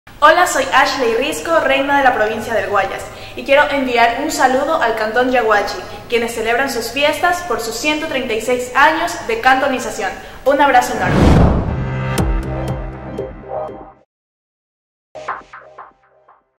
ASHLEY RISCO SALUDA A YAGUACHI 136 AÑOS
La reina de reinas saluda a la tierra que la vio nacer, crecer y convertirse en una mujer exitosa.